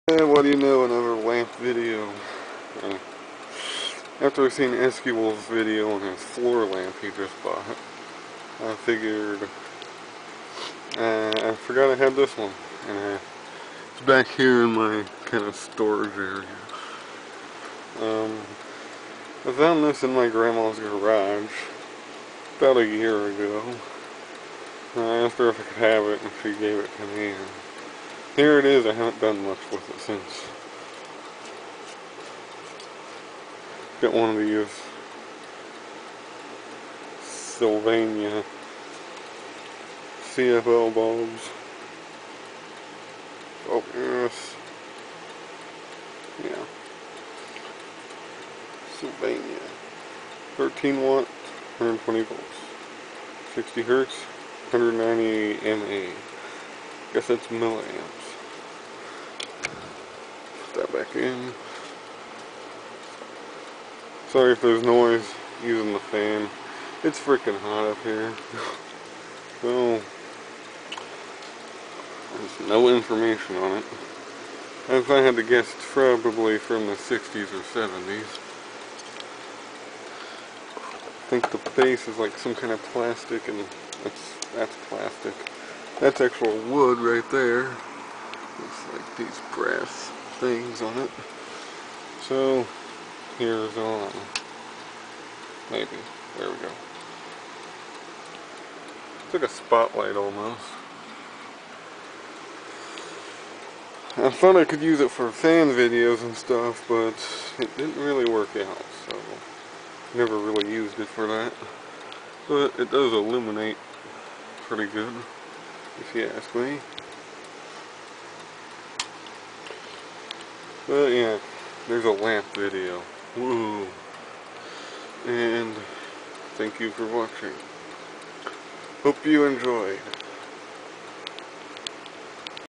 Anther Vintage Lamp.
heres a 1960's or so table lamp. not much to say about this. but it is really neat IMO.